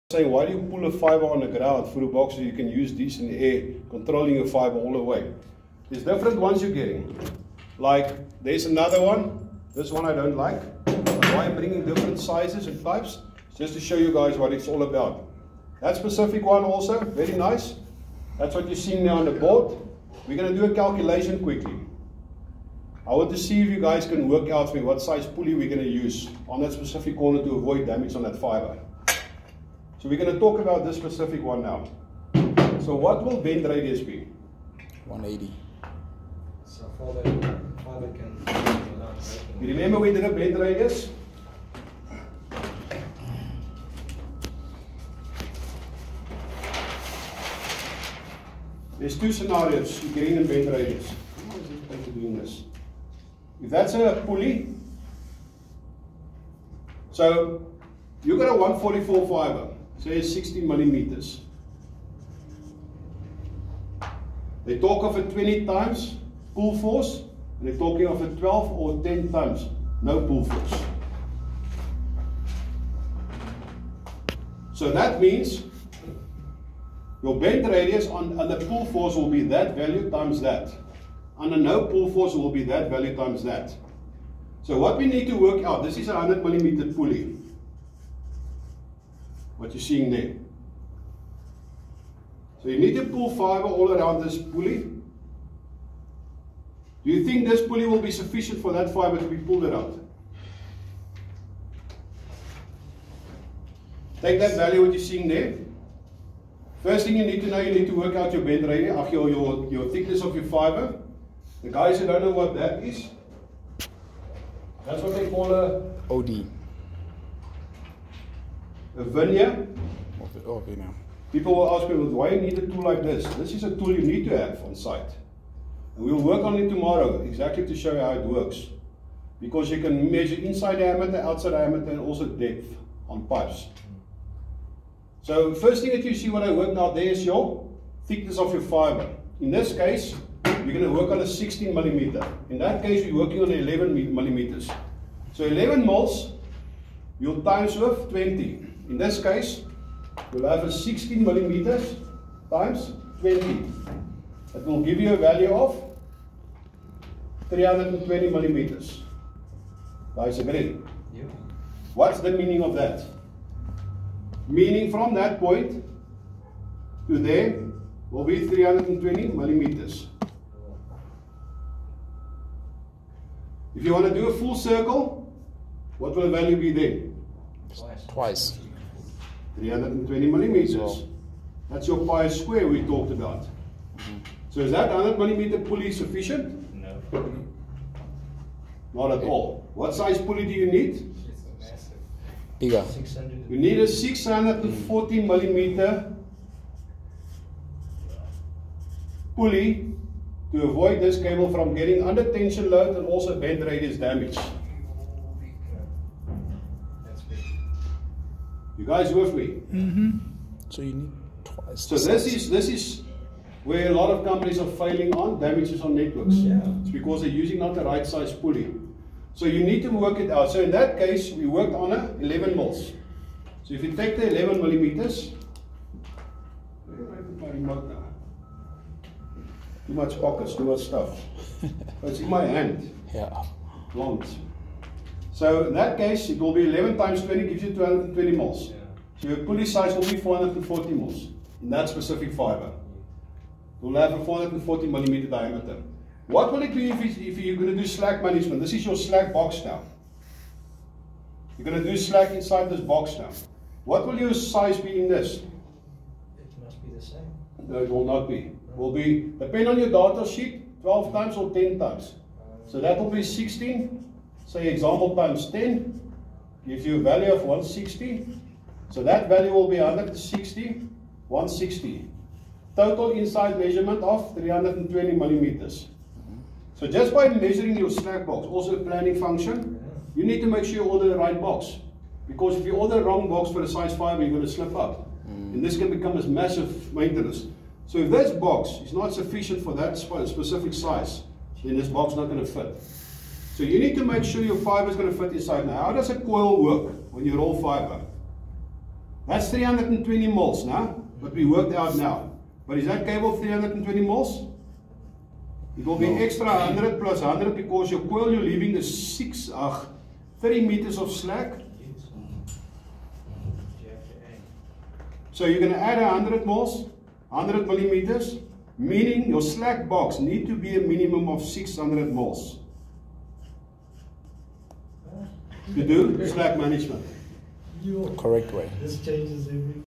Calculate Bend radius on Fibre Cable under Pull force.
Shared Pole route training session explaining how to calculate a bend radius on a fibre cable. The video explain how to calculate the correct size pulley needed to pull the fibre cable around a bend. Not using the correct size pulleys around a bend under pull tension will cause damage to the fibre cable.The second part of the calculation is to work out what size slack box will be require, to accommadate the 30m of fibre cable that will be coiled into the box (no pull force)